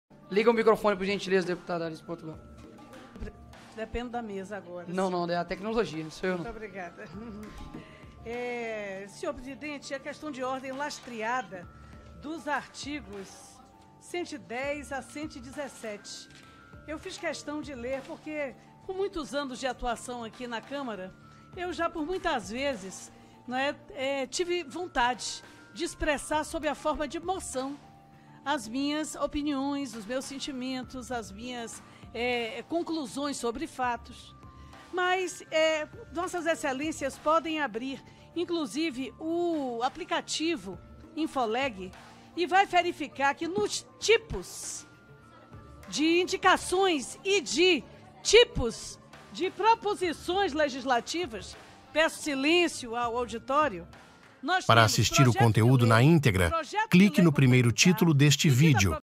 TRETA! ALICE PORTUGAL X NIKOLAS FERREIRA 🔥🔥🔥
DEPUTADA ALICE PORTUGAL CAUSA TUMULTO NA COMISSÃO DE EDUCAÇÃO, VEJA!!!